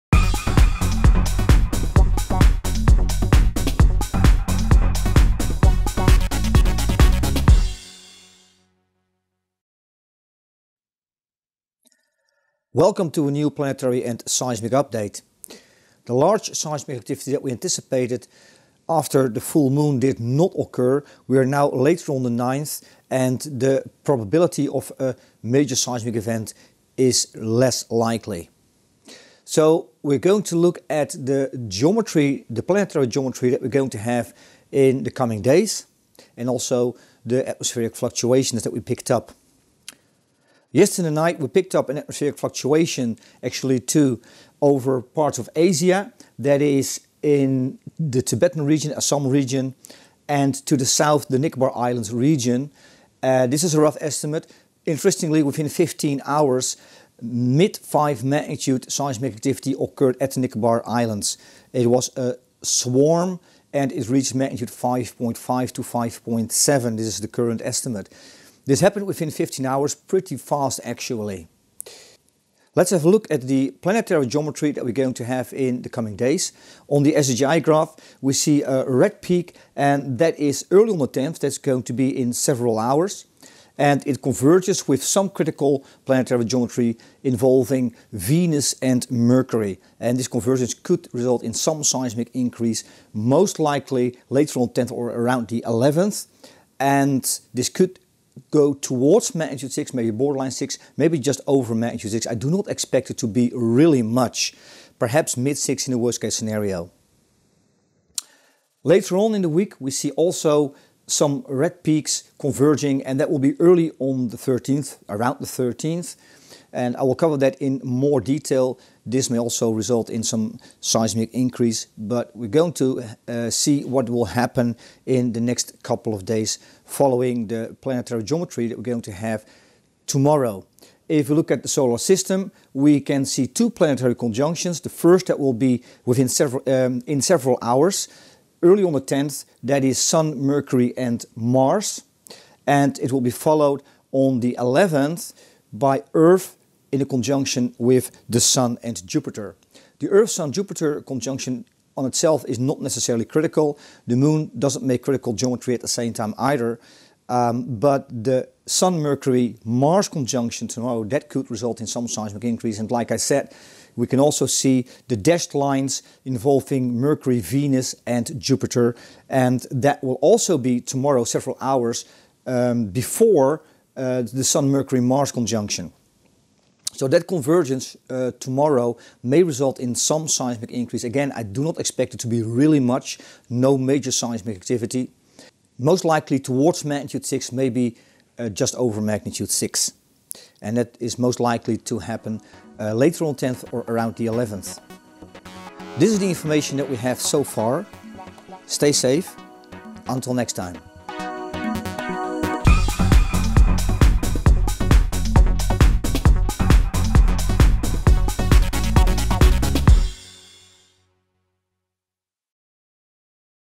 Planetary & Seismic Update 9 April 2023
A convergence of specific planetary geometry on 10 April may result in some seismic increase, possibly reaching borderline 6 magnitude.